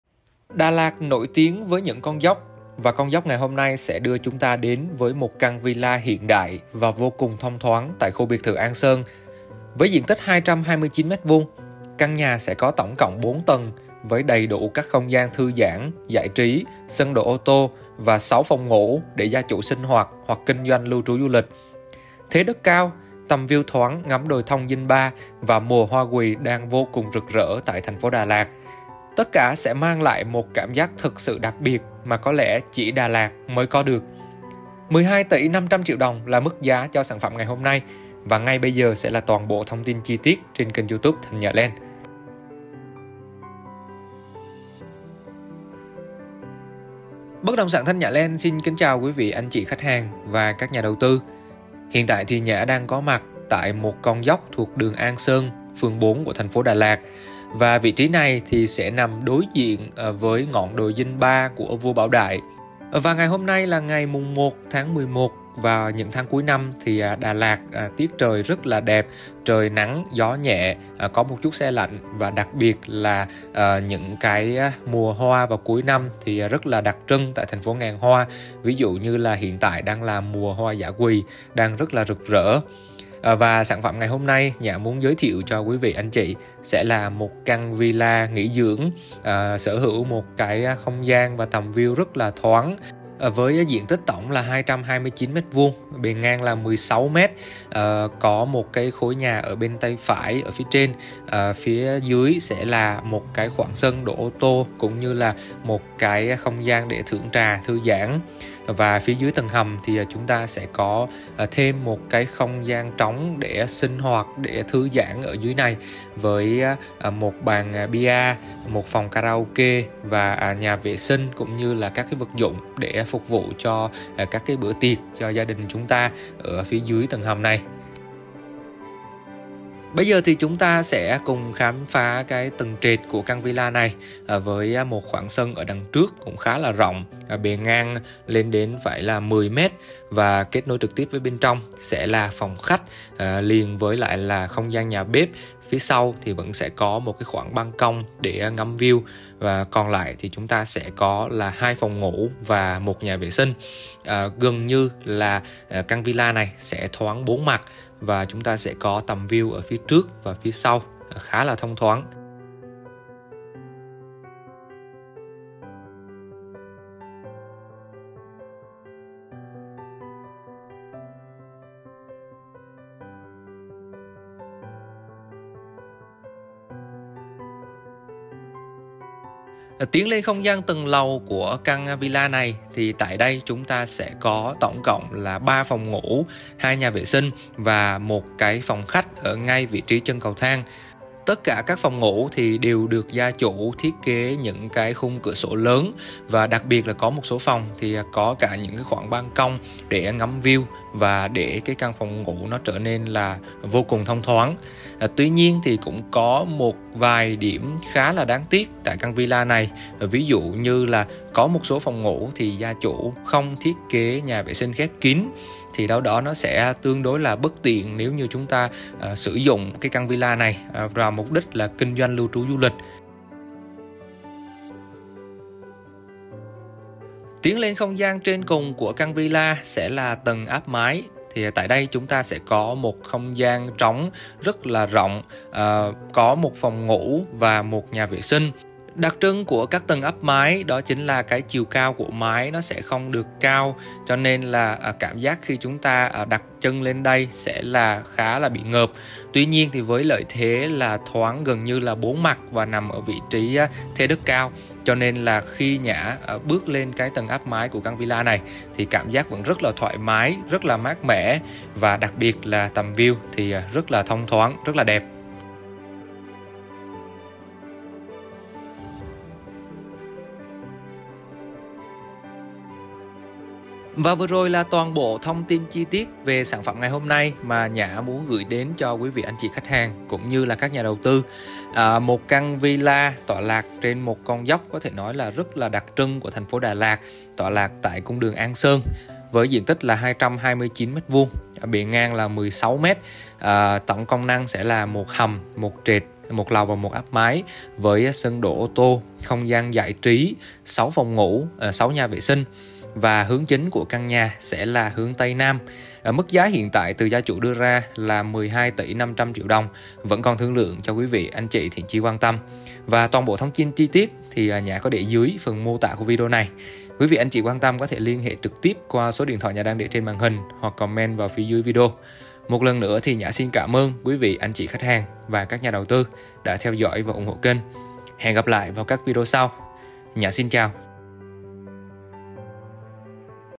Villa An Sơn - Những đặc trưng của Đà Lạt
✨🏡✨ Bán Villa Hẻm ô tô lớn đường An Sơn, Phường 4, Đà Lạt
👉 Diện tích: 229.26m2  (thổ cư 201m2 , đất nông nghiệp 27.36 m2), ngang 16m
👉 Kết cấu:  4 tầng - 6 phòng - 6 WC
👉 Quy hoạch: Biệt lập
👉 Pháp lý: Sổ riêng
👉 Hướng: Tây Nam
👉 Giá bán: 12,5 tỷ